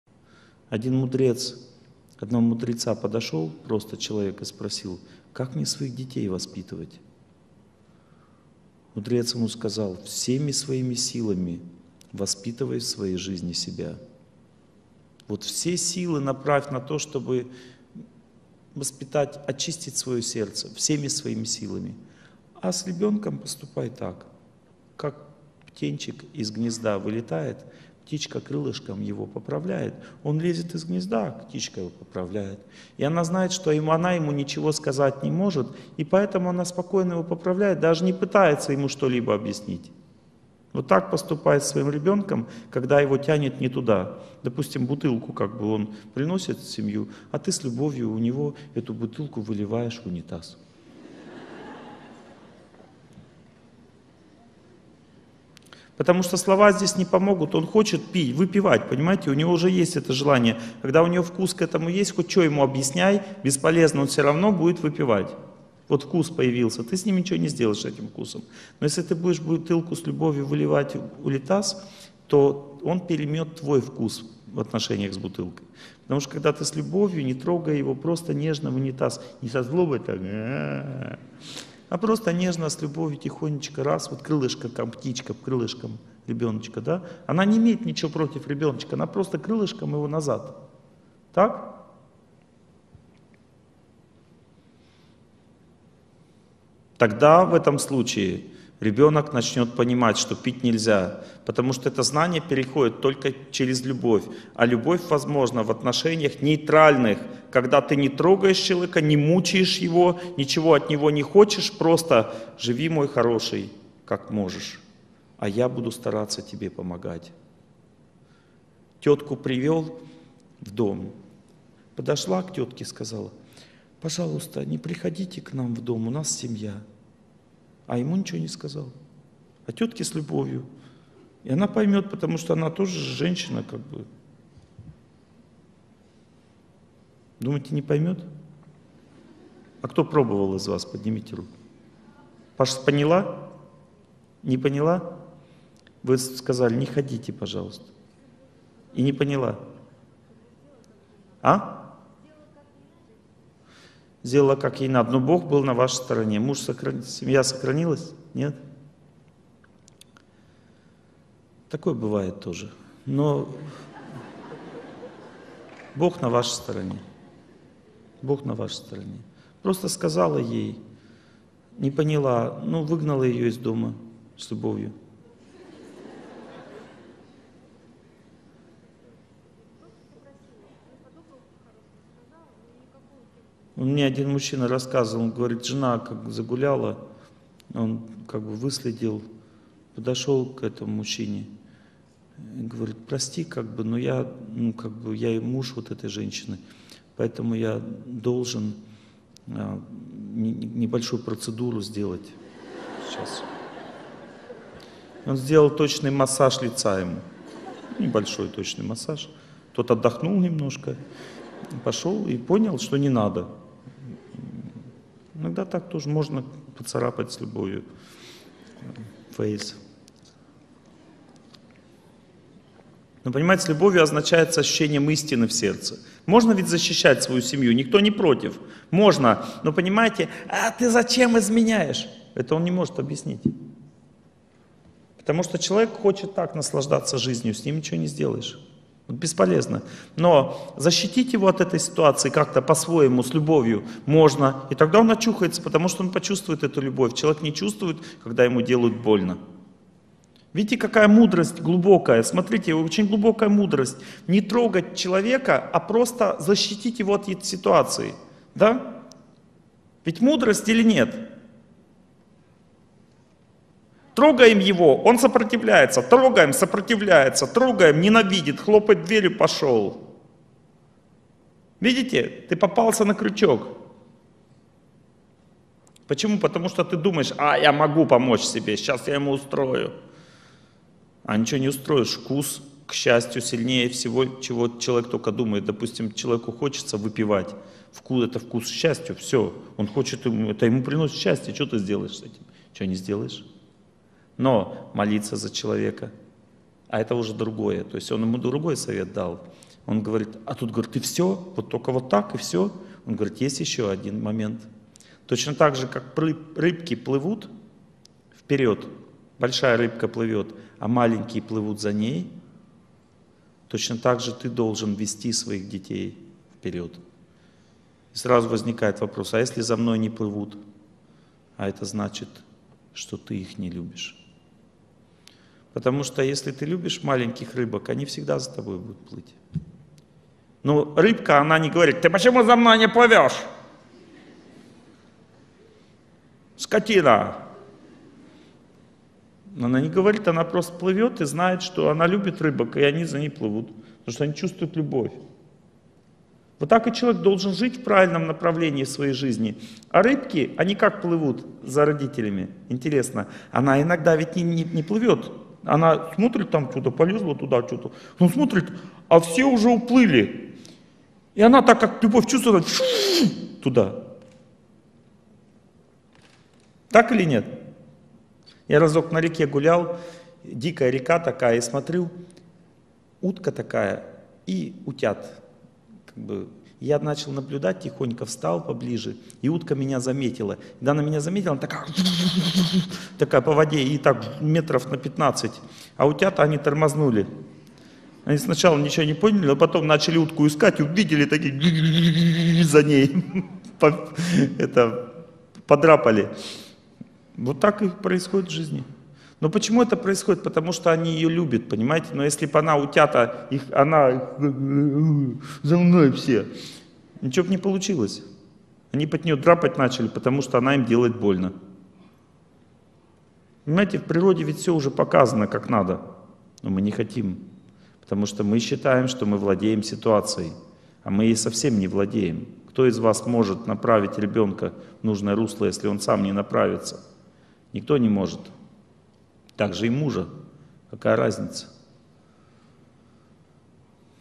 Торсунов О.Г. О воспитании
В этом видео говорится о воспитании
Из леции "Победа над тяжелой семейной кармой" 2012